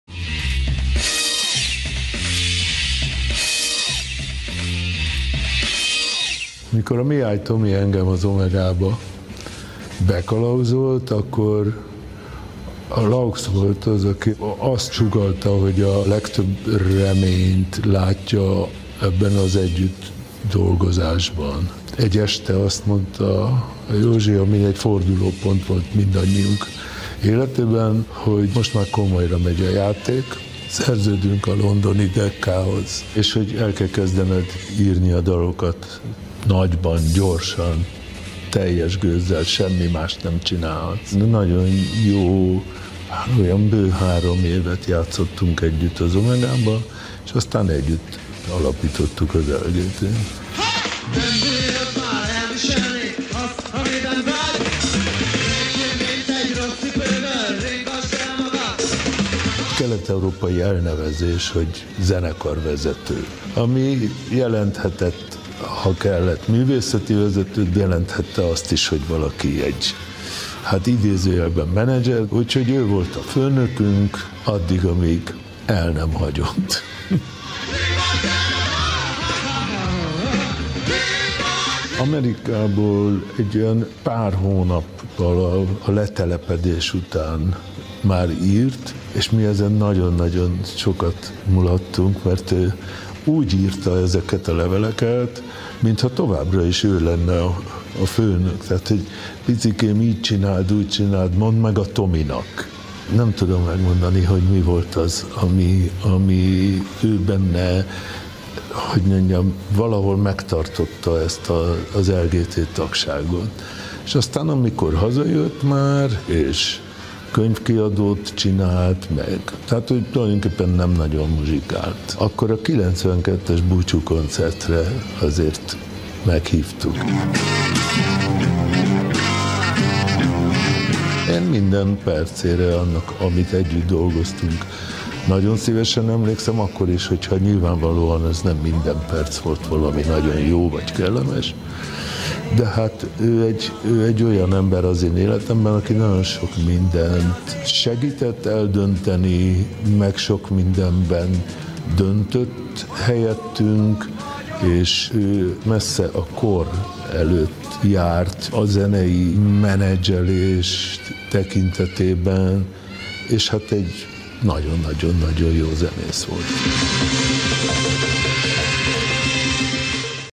Presser Gábor emlékei Laux Józsefről
Laux József 1971-ben megalapította a Locomotiv GT-t, amely szintén az ország egyik legismertebb együttese lett. Alapítótársa, Presser Gábor stábunknak mesélt arról, hogyan is emlékszik vissza erre az időszakra.